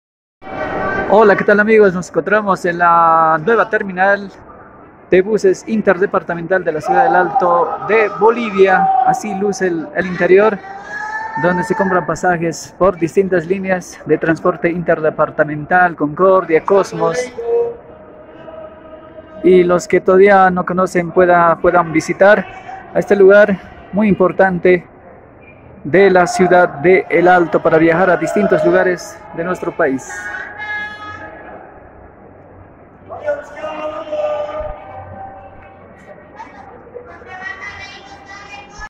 MODERNA TERMINAL DE BUSES DE LA CIUDAD DE EL ALTO 🚌🚗🇧🇴
Ubicada en zona Bolívar D, del Distrito 2 de la ciudad de El Alto, sobre la avenida Ladislao Cabrera que conecta con camino a Viacha.